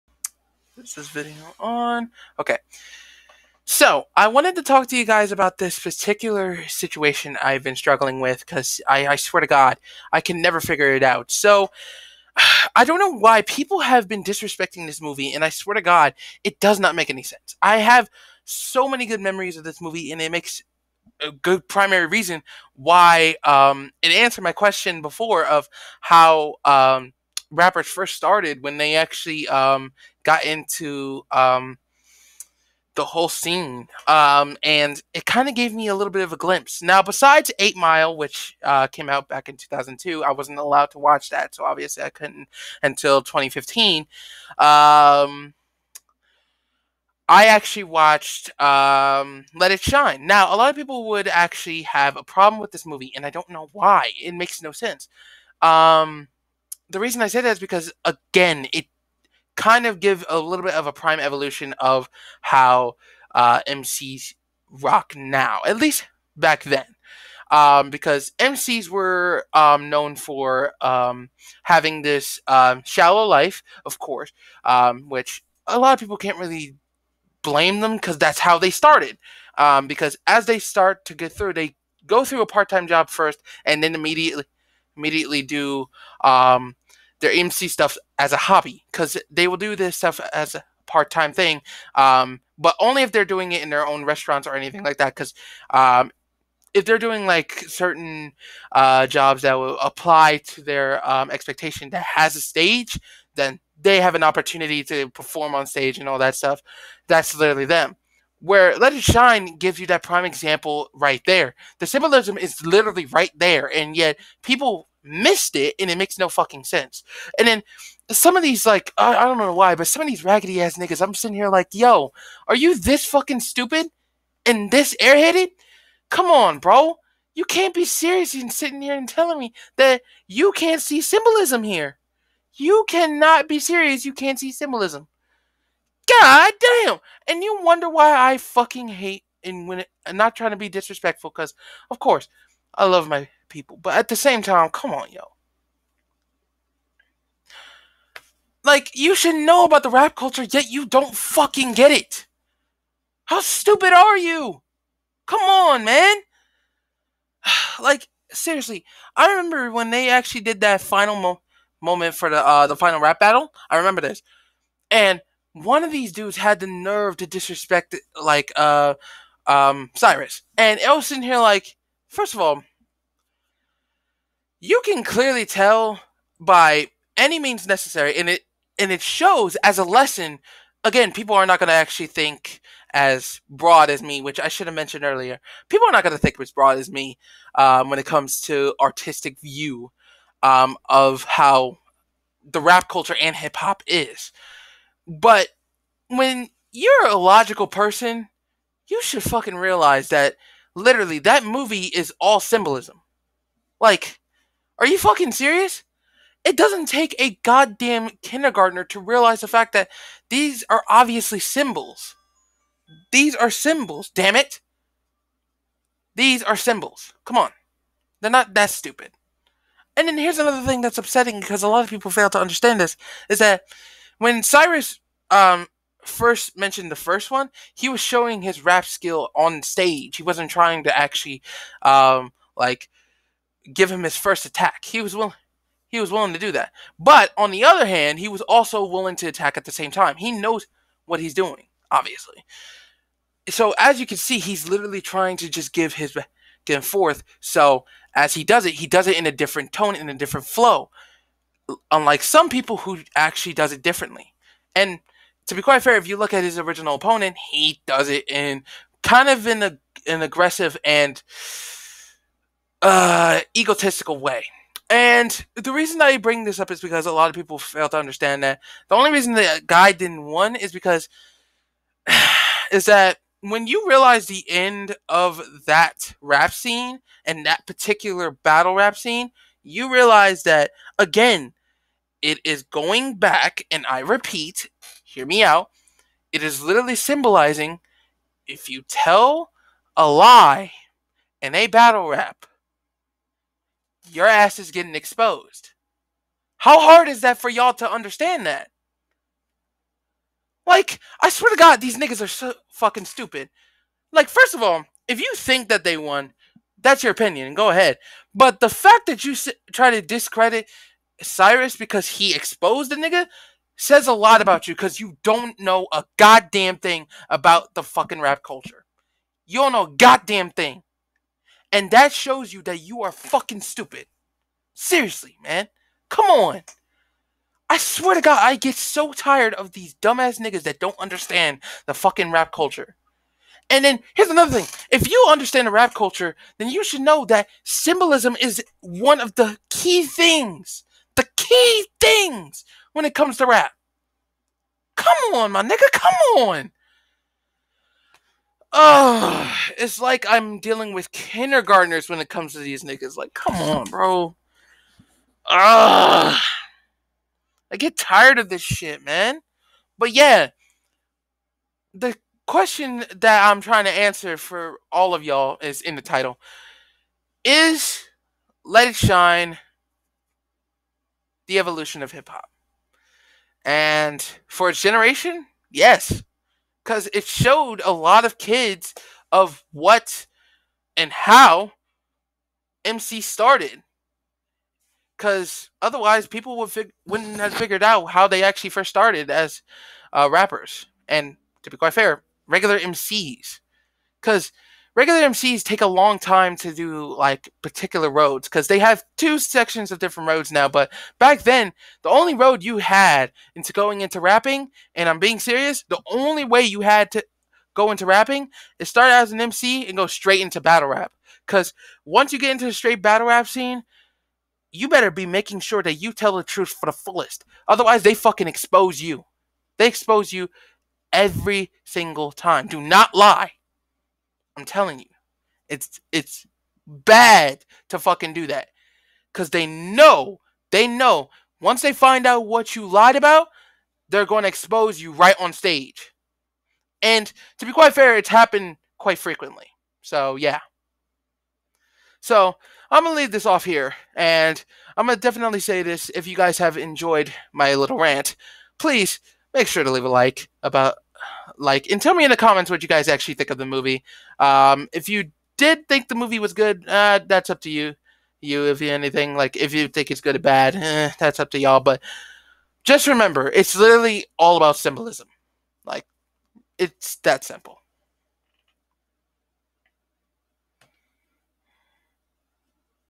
Is Let it Shine the modern Evolution of hip hop? (Rap and Movie Rant)
Short rant about certain who took that movie for granted.